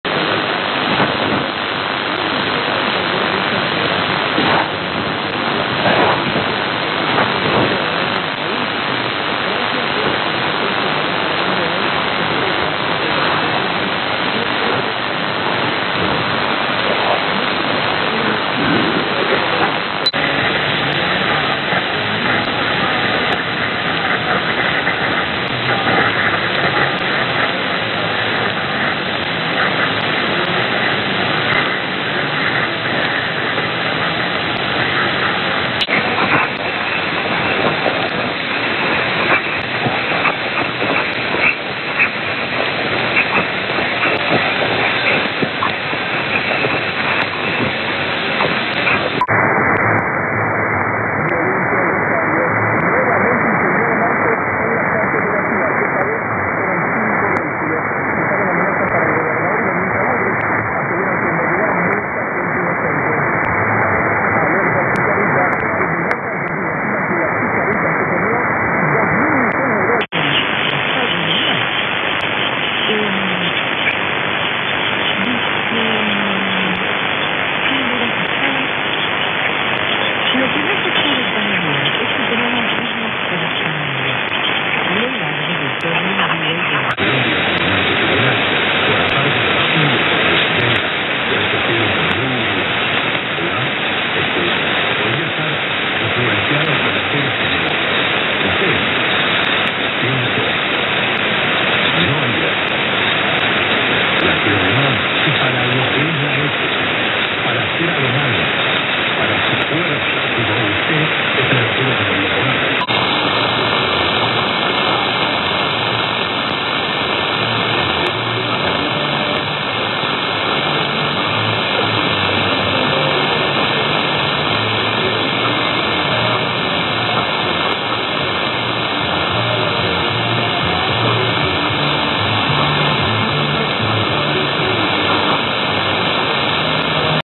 Quick MW Bandscan - 2024-05-04
Last night brought many signals South America. Sadly, all too short and too weak for identification. 1510 is most likely Radio Monumental from Quito, Ecuador. Any thoughts about the other stations?